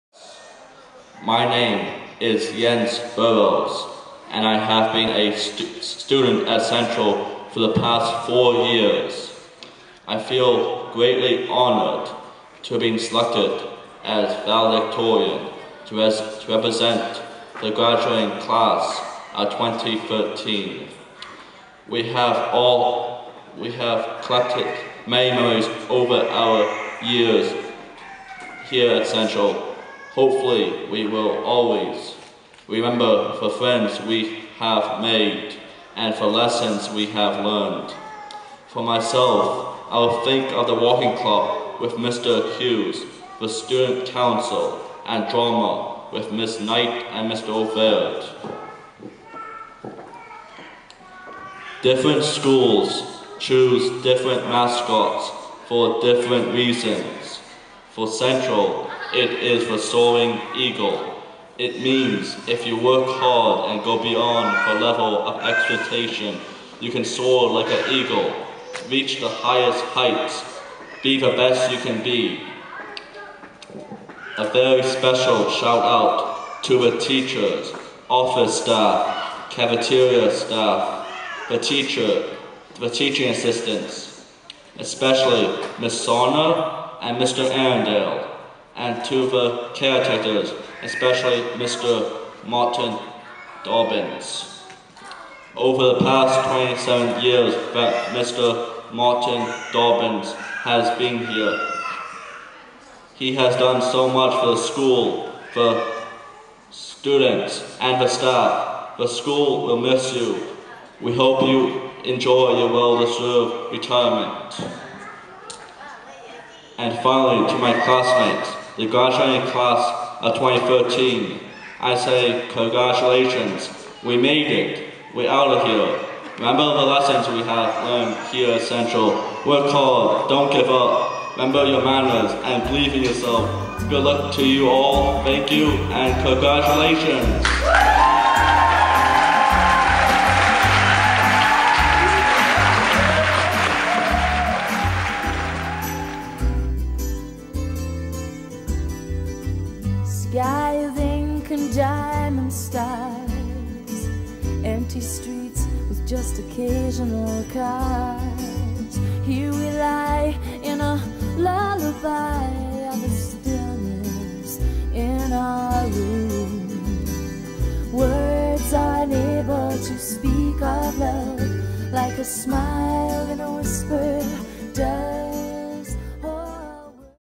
Jens The Valedictorian
Jens Burrows is Valedictorian and top award winner at Etobicoke Central High School, Etobicoke, Ontario, June 26, 2013.  Congratulations Jens!